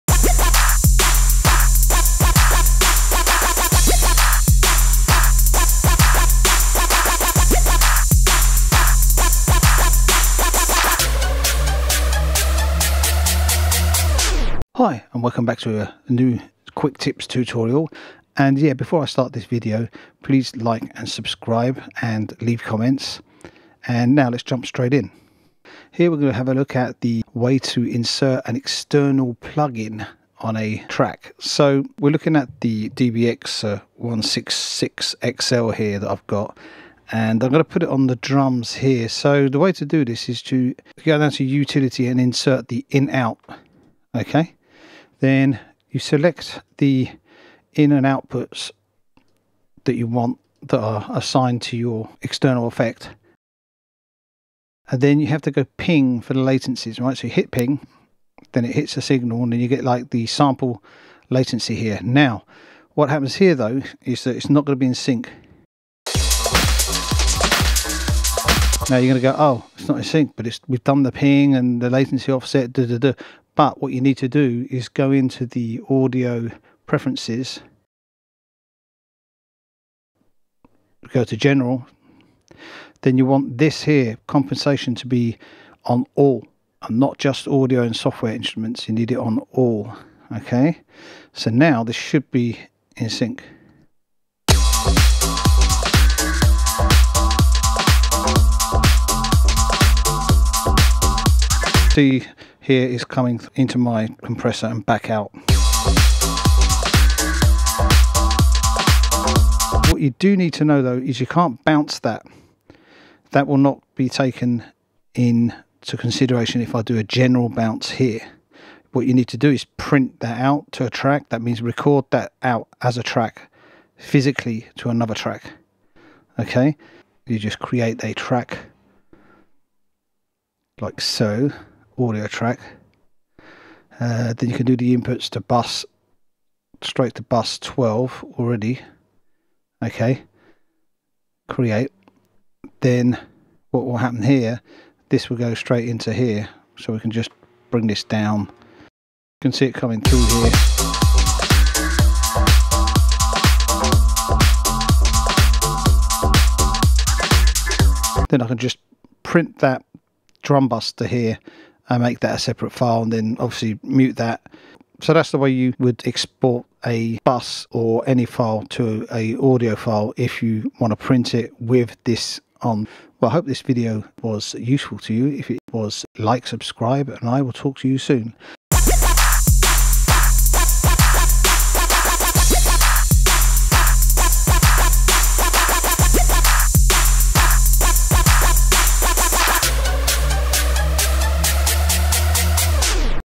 EXTERNAL EFFECTS INSERT In Logic pro X
Here i take a look at the utility I/O to use an external effects unit
i use a DBX 166 XL as an insert in logic on a drum bus.
Then print it a s a trac